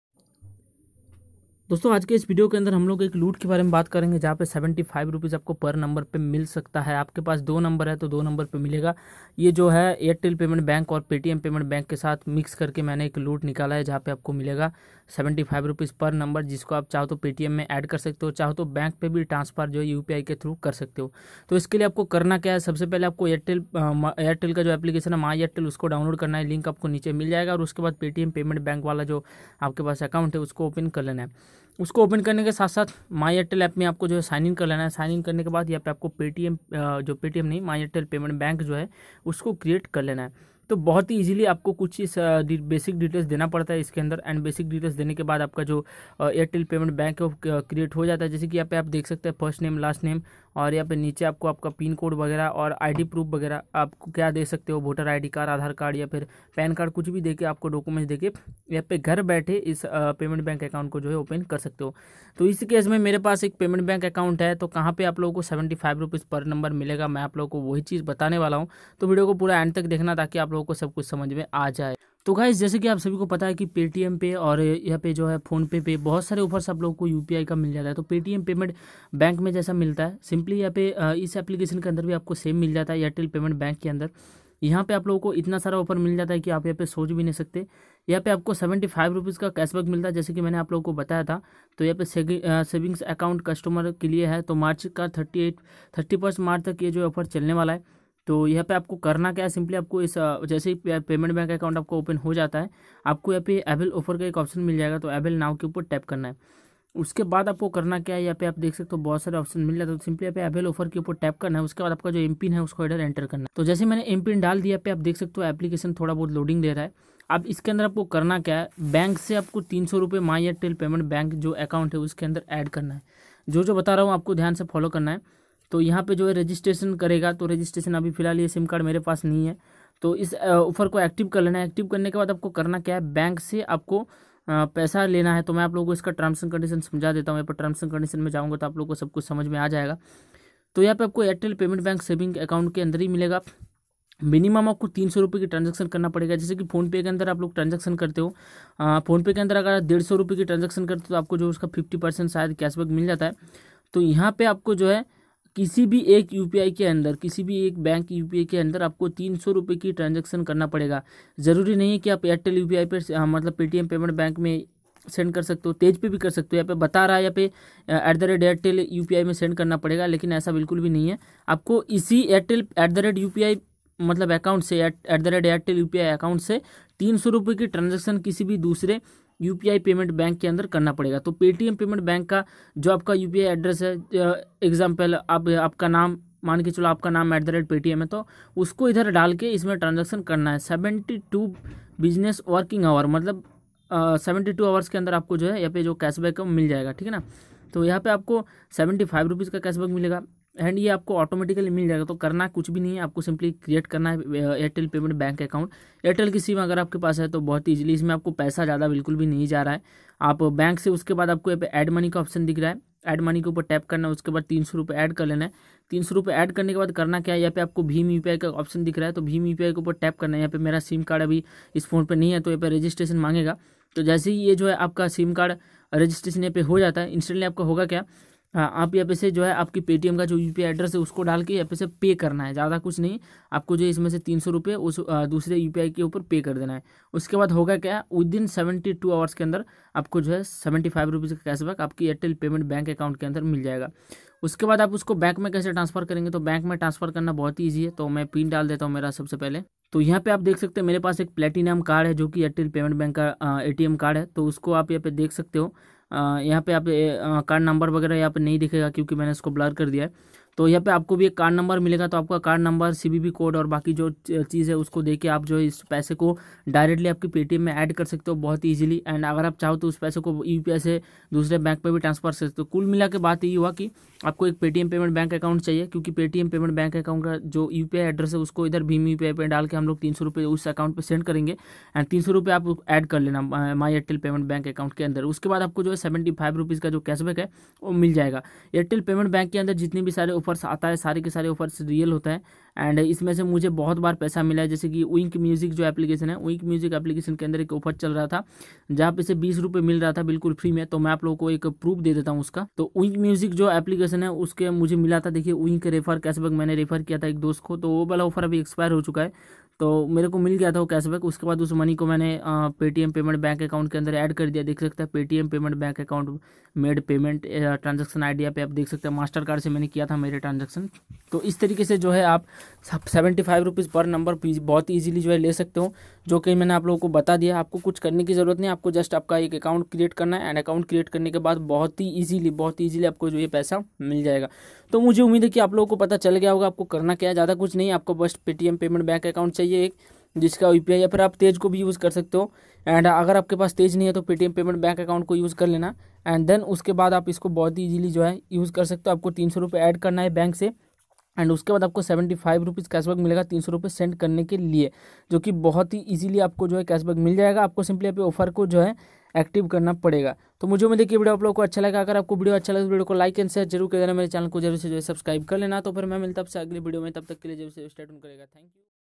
Paytm Payment Bank + Airtel Payment Bank Loot Offer ⏩ ₹75 Per Account ⏩(Offer Expired)
Hello Friends It's New Loot Offer ₹75 Per Account Full Tutorial Added Must Watch The Video.

Download This App And Get ₹5000 Free Paytm Cash ₹120 Per Refer